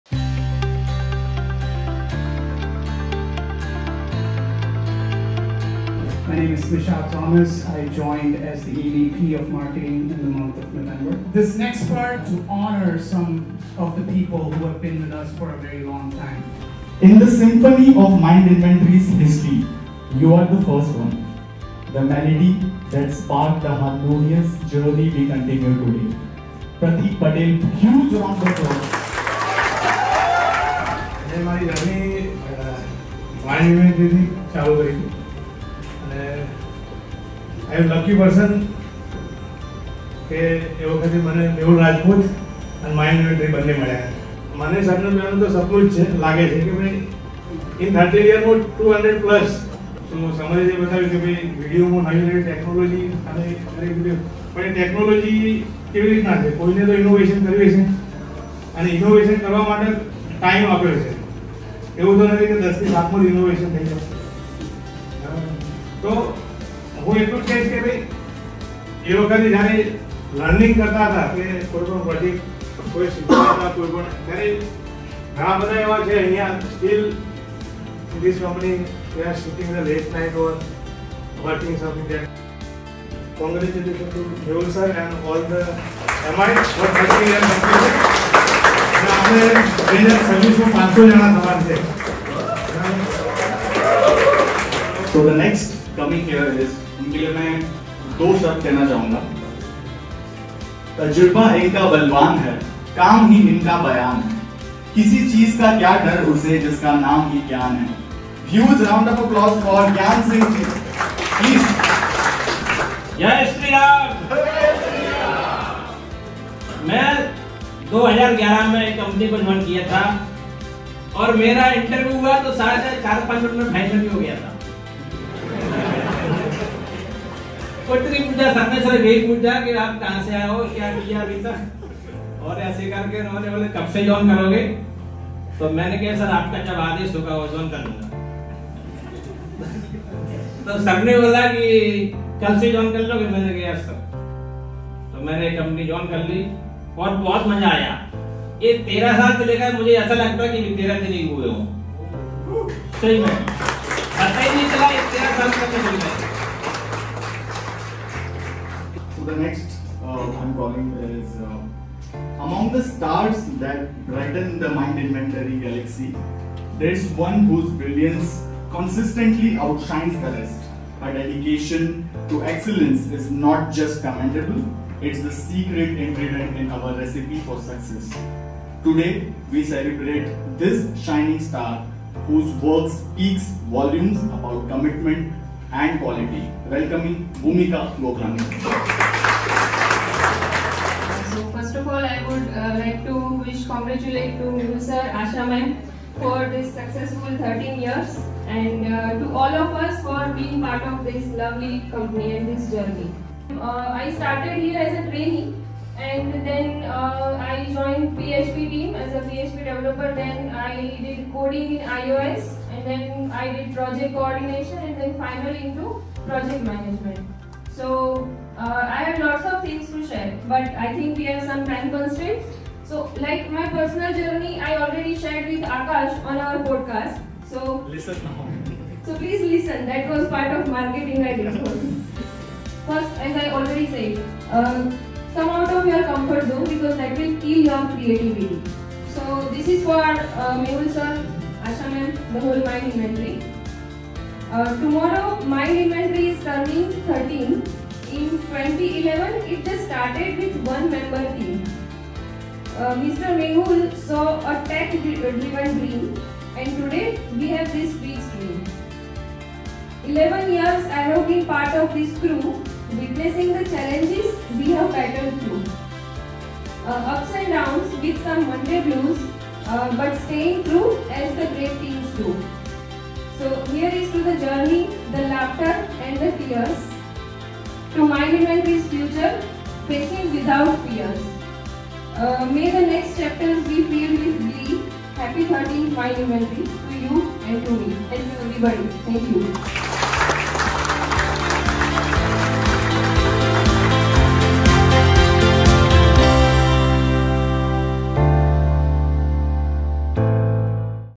Celebrating a Decade of Dedication | Employee Stories
Witness the stories of resilience, dedication, and growth as our long-serving employees share their invaluable experiences at MindInventory.
From a decade of milestones to marching ahead with unwavering commitment, their journeys inspire us.

MindInventory - Your New Age Digital Transformation Partner!!